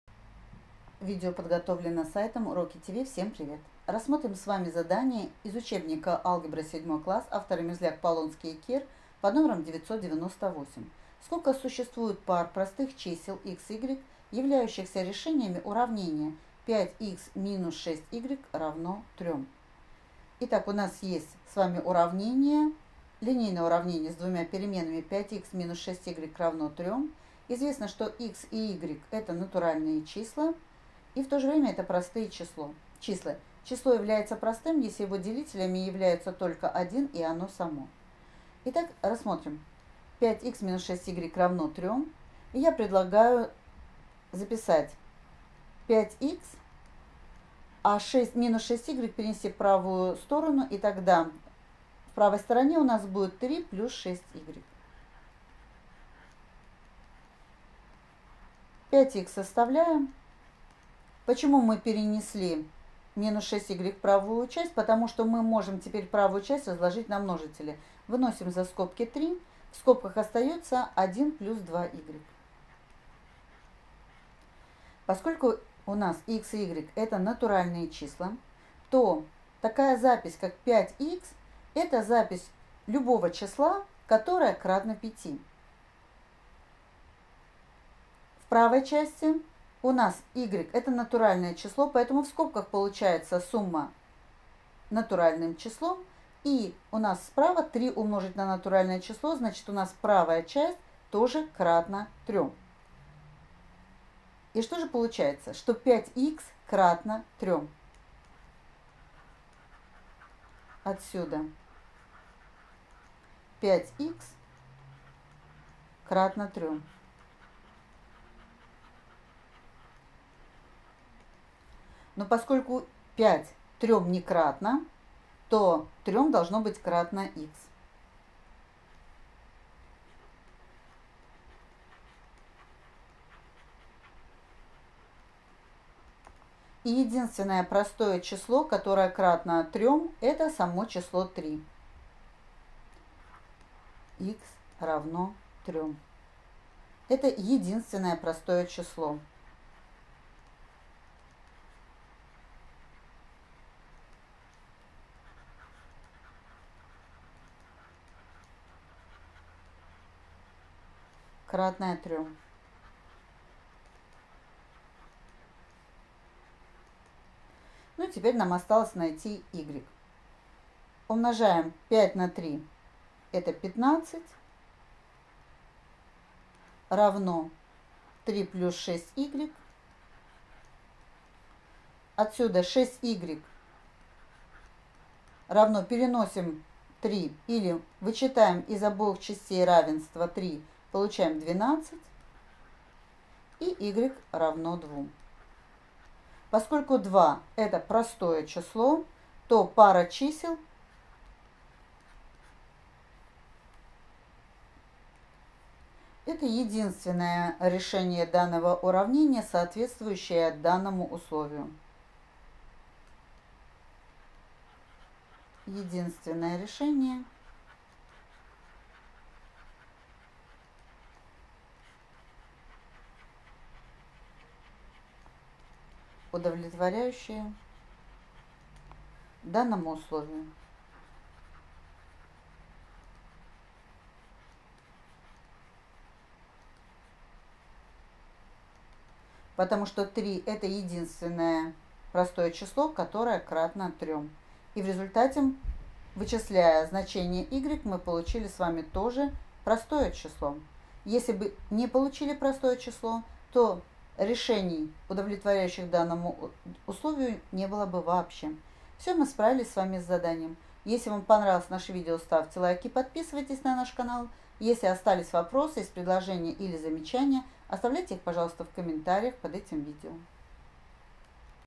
Задание №998 - ГДЗ по алгебре 7 класс (Мерзляк А.Г.)
Чтобы выполнить задание № 998, в котором требуется определить сколько существует пар простых чисел являющихся решениями данного уравнения, нужно знать материал §25 4 главы, расположенный на страницах 186-189. А также вспомнить материал §23 3 главы и определение простого числа.